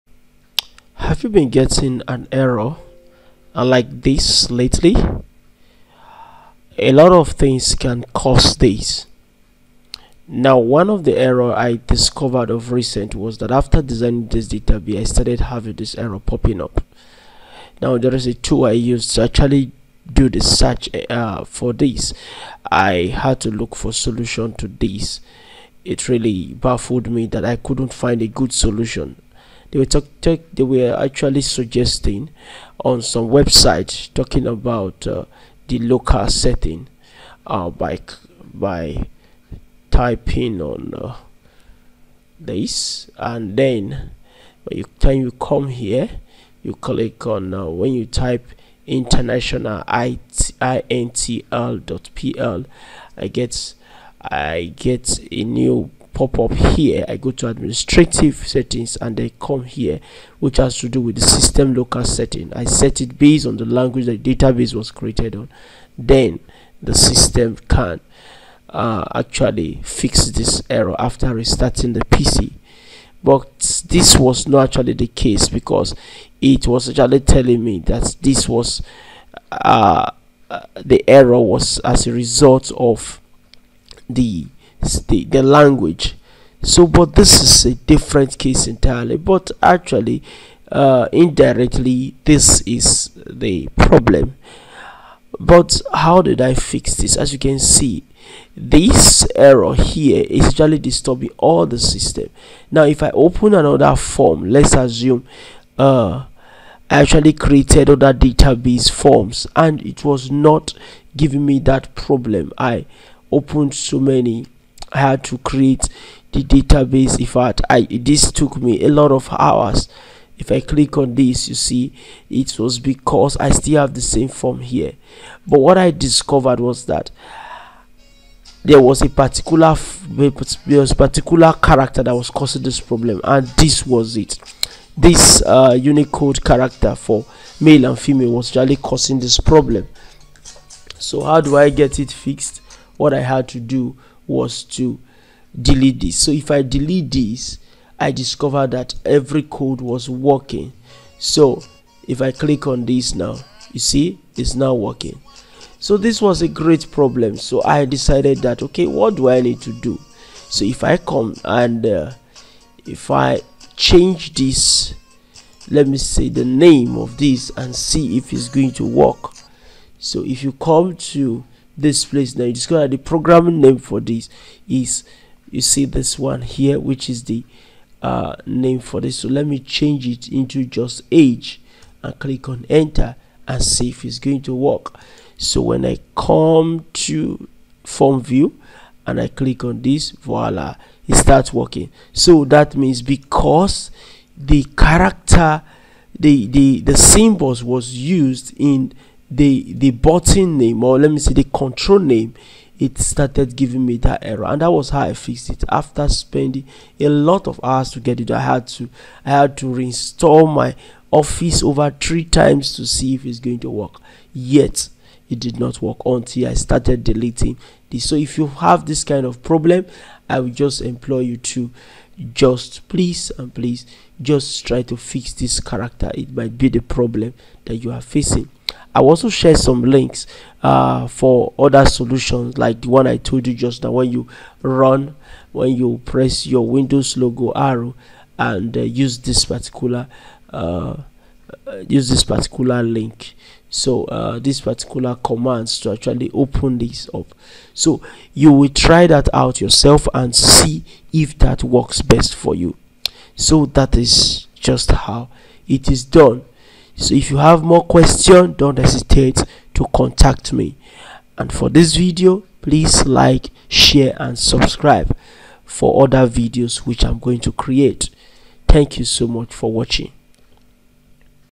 A problem occurred while Microsoft Access was communicating with the OLE server or ActiveX Control
This error will stop you from continue writing your codees